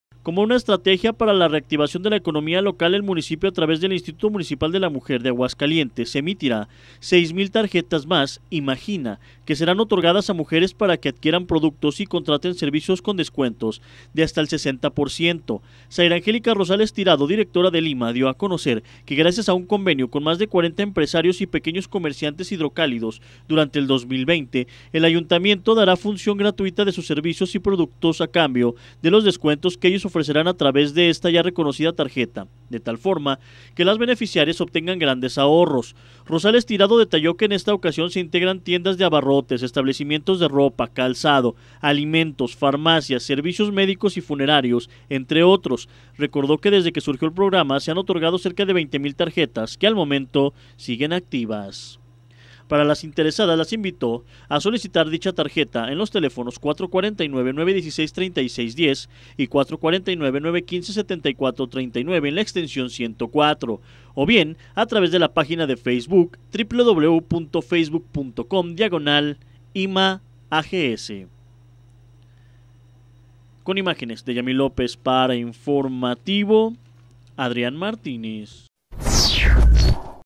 TARJETA DE DESCUENTOS PARA MUJERES
El Instituto Municipal de la Mujer de Aguascalientes emitirá 6 mil tarjetas para que mujeres obtengan descuentos de hasta el 60 por ciento en distintos productos.